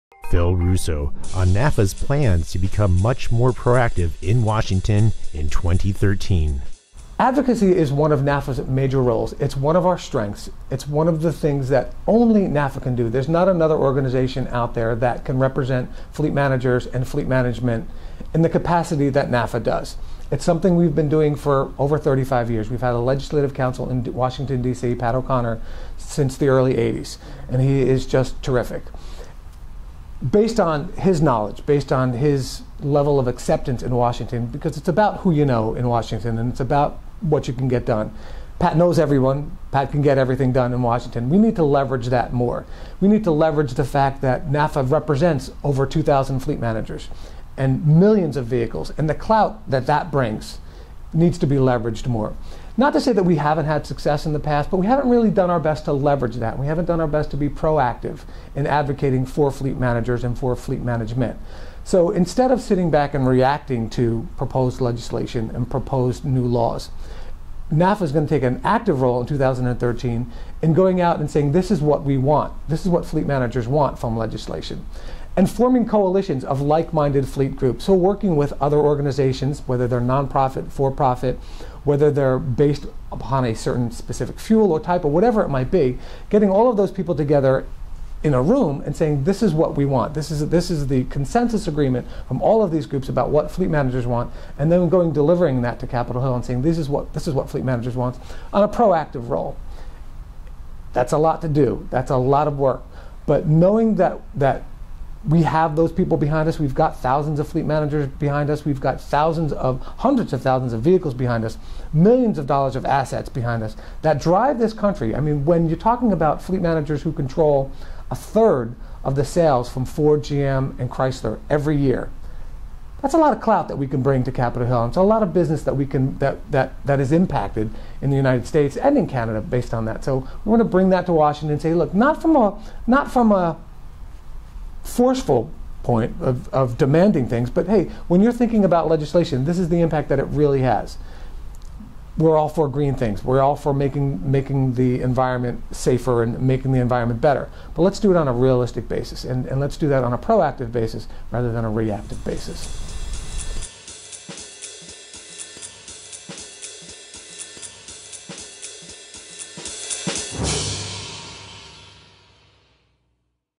NAFA's Approach to Legislative Advocacy | Phillip Russo | Automotive Digest
Phil Russo, Executive Director of NAFA, on NAFA's plans to become much more proactive in Washington in 2013.

Ain_10_16_12_IntFleetMgmtSem_ NAFA_Phillip_Russo_VC5.mov